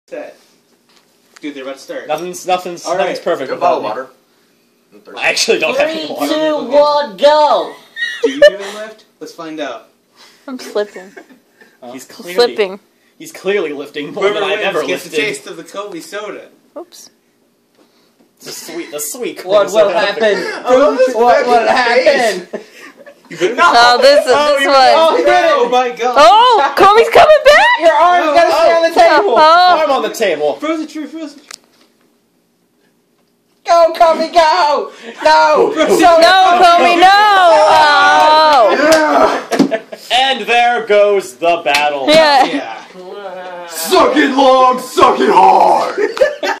FIGHT ME @ PAX TIEBREAKER! - KomiExplosion vs FrozenTrain (Arm Wrestling Match #2)
Sam (he was recording and talking)
UltimateHero13 (he was the mexican in the mirror)